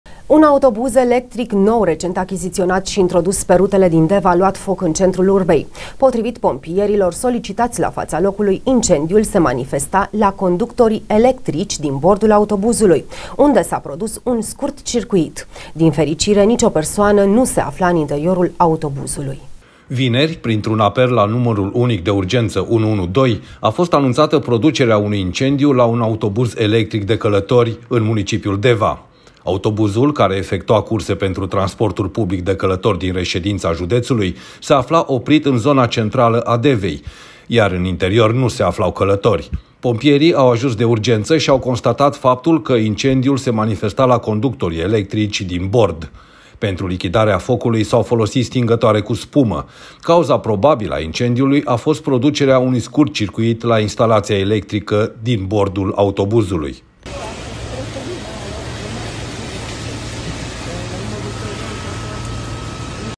INCENDIU LA UN AUTOBUZ ELECTRIC, ÎN DEVA
POMPIERII AU INTERVENIT DE URGENȚĂ. INCENDIUL SE MANIFESTA LA CONDUCTORII ELECTRICI DIN BORD
 Un autobuz electric nou, recent achiziționat și introdus pe rutele din Deva, a luat foc, în centrul urbei. Potrivit pompierilor solicitați la fața locului, incendiul se manifesta la conductorii electrici din bordul autobuzului, unde s-a produs un scurtcircuit. Din fericire, nicio persoană nu se afla în interiorul autobuzului.